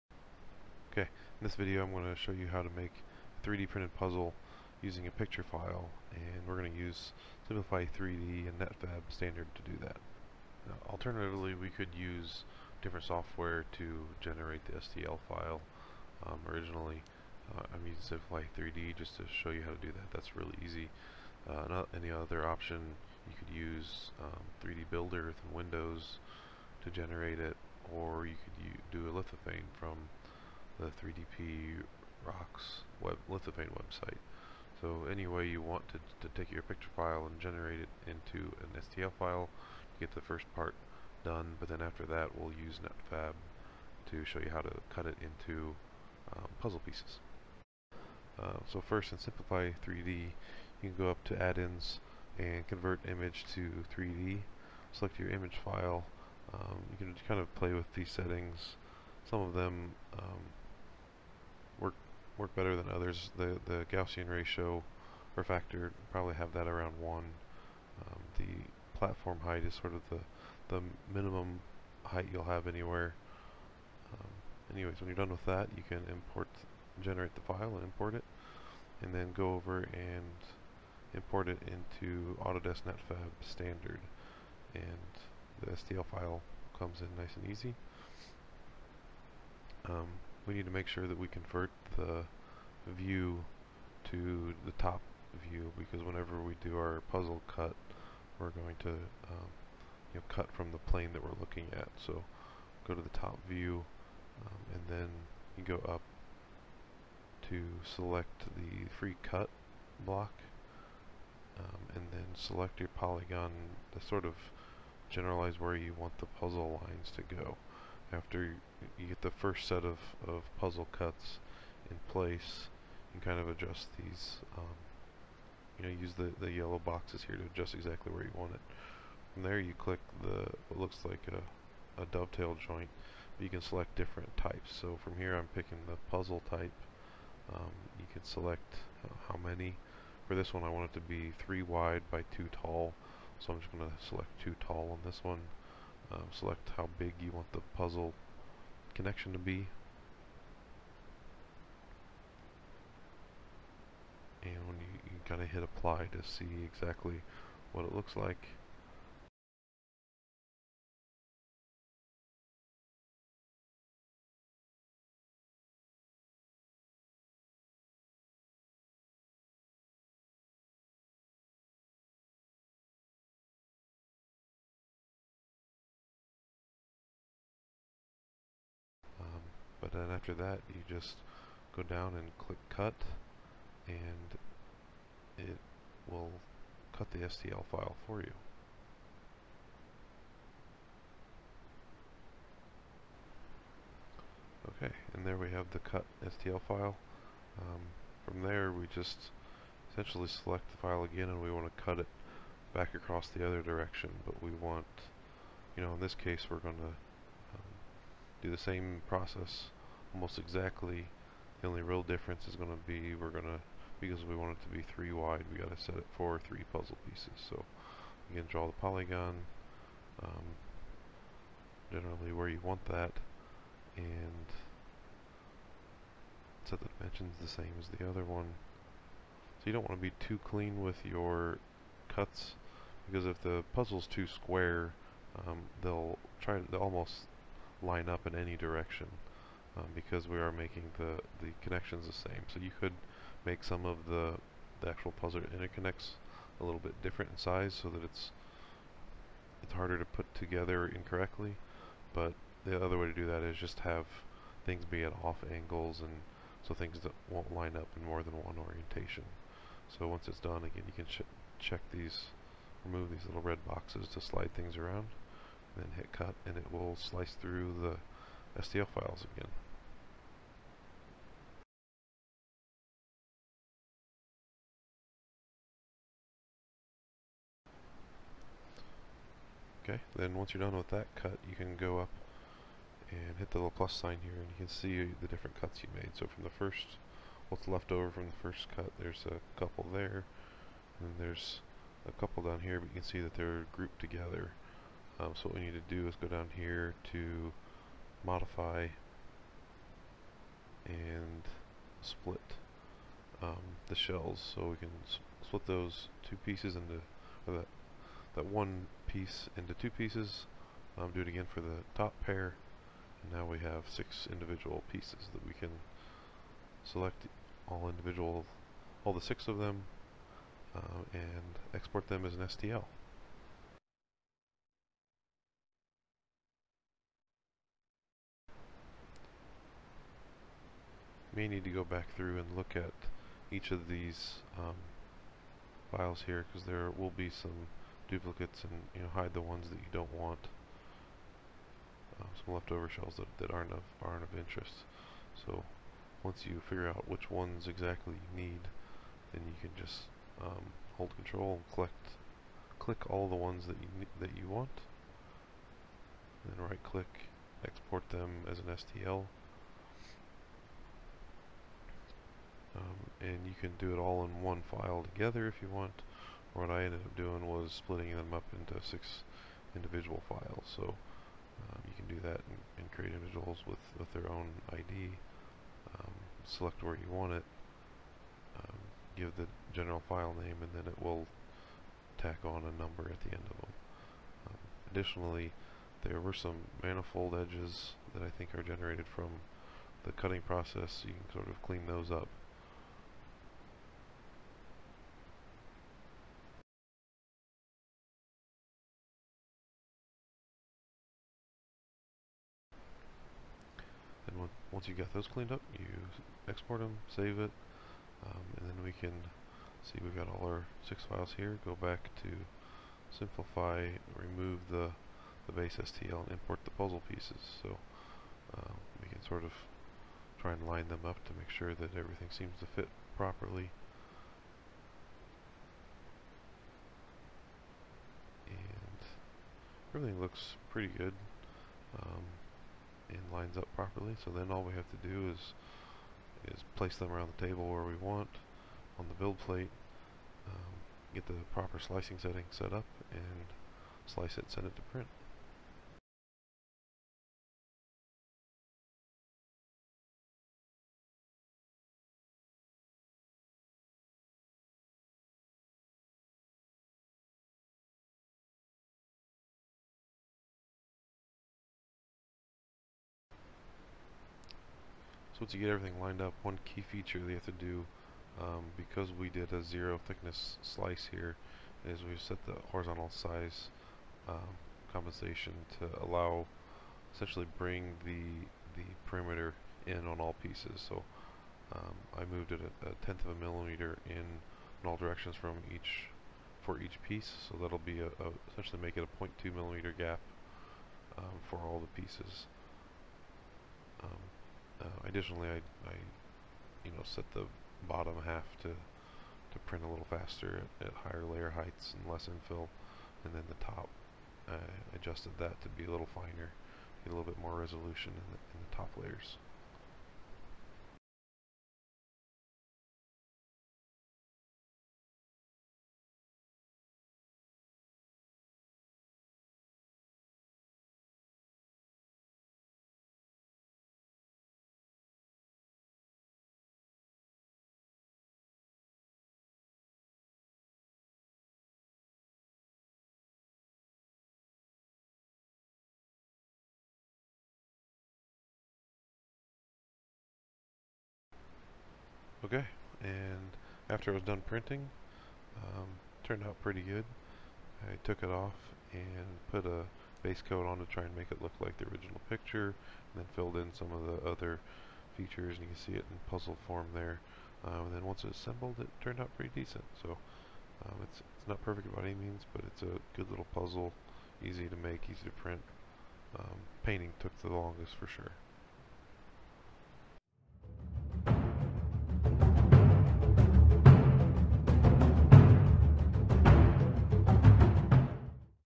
Make your own 3d printed puzzle from a picture (using Netfabb and Simplify3d)
In this video, I demo how to create a 3d printable puzzle from an image file.
First, I show how make an stl from an image file using simplify3d, though you could use 3Dbuilder (windows) or create a lithophane
Next, using Autodesk Netfabb Standard (free with .edu address) I'll show how to cut it into puzzle pieces.
Lastly, I print out a puzzle, paint it, and put it together.